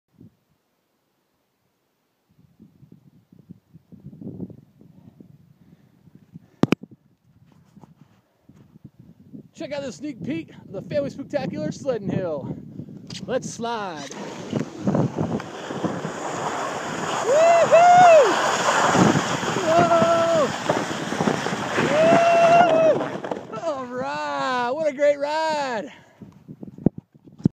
Warm weather sledding hill at Family Spooktacular
Warm weather sledding for the Family Spooktacular at Camp Alexander.